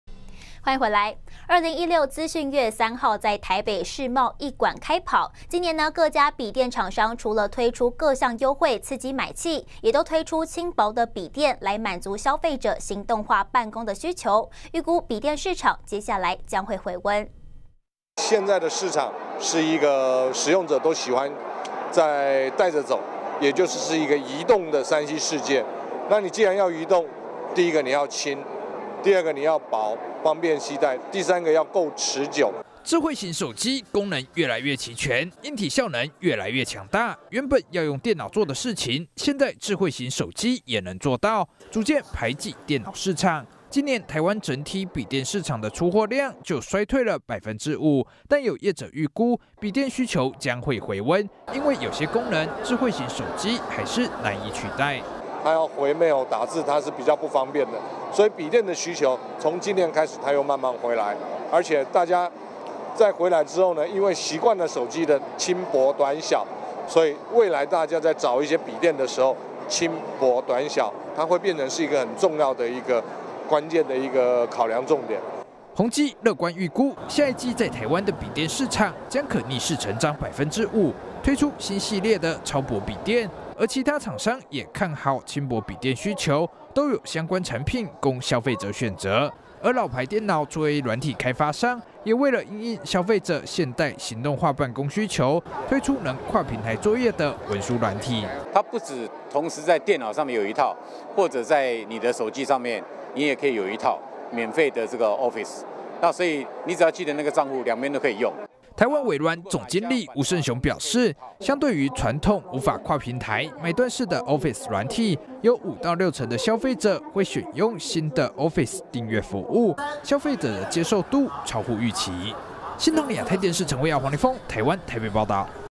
【新唐人/NTD】筆電市場將回溫？ 推輕薄型滿足行動需求｜資訊月｜世貿一館｜資訊展｜筆電｜微軟｜宏碁
2016資訊月3日在台北市貿一館開跑，今年各家筆電廠商除了推出各項優惠，刺激買氣，也都推出輕薄筆電，滿足消費者行動化辦公的需求，預估筆電市場，接下來將回溫。